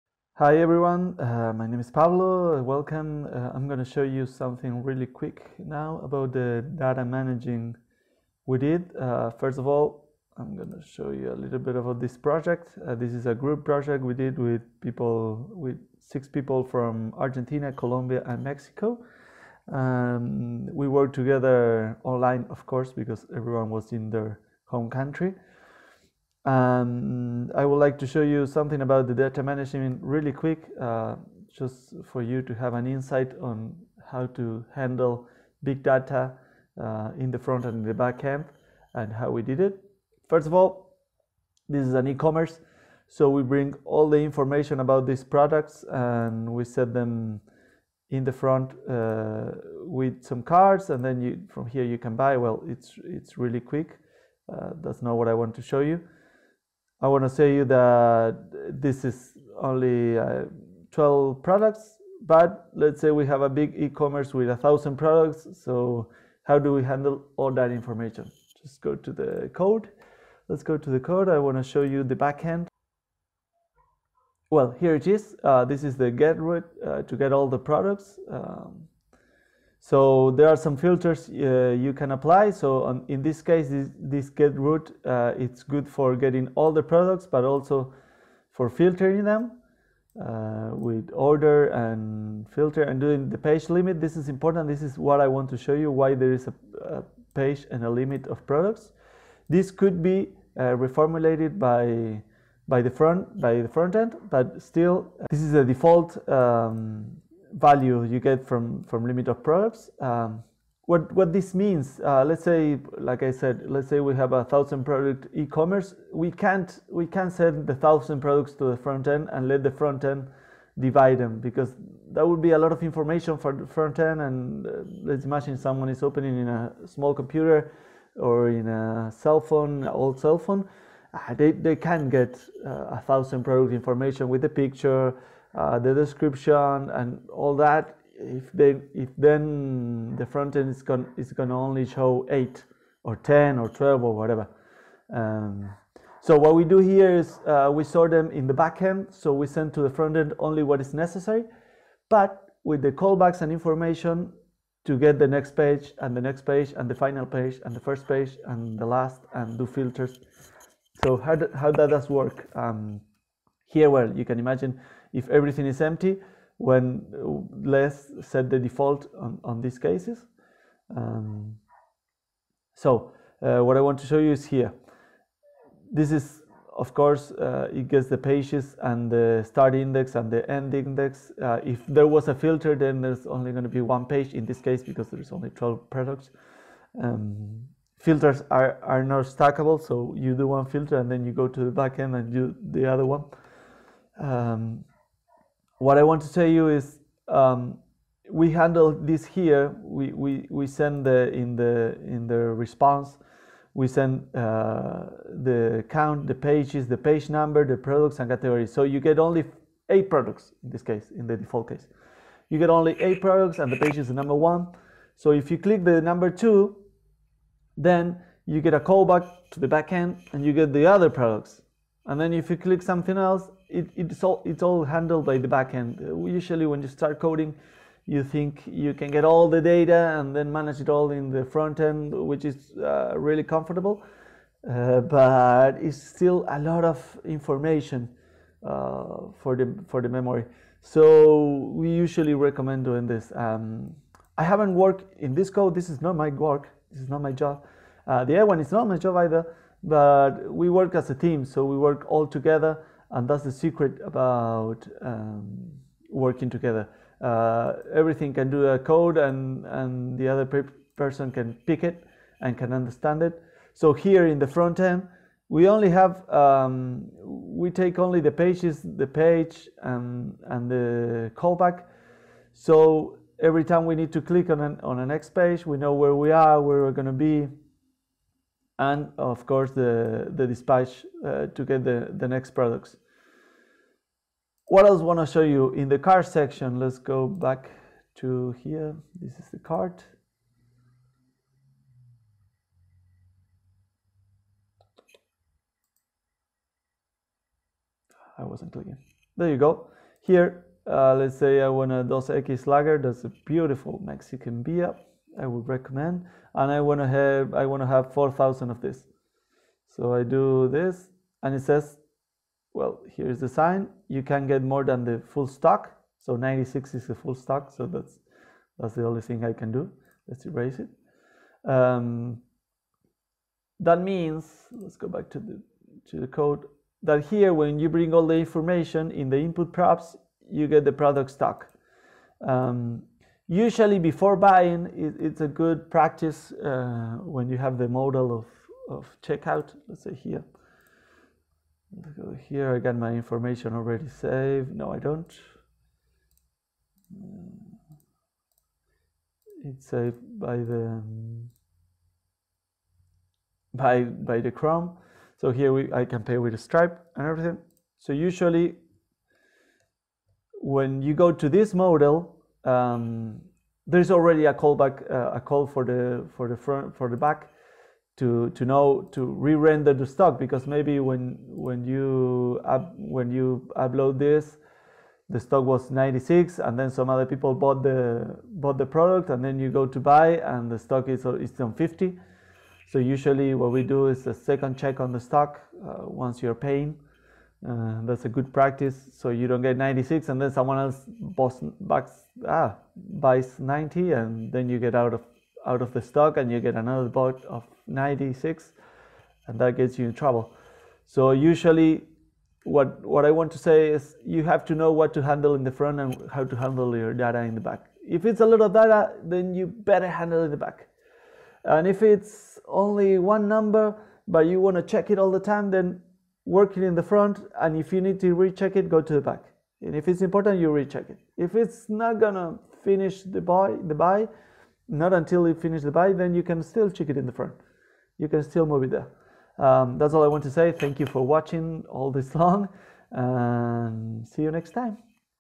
Backend to Frontend, where to sort and filter?
Just a quick overview of a group project and why is better to sort and filter in the backend. Your app could be handeling few products now, but it's better to think of it scalable, what would happen if 2.000 products are send each time from the back to a front that only displays 8?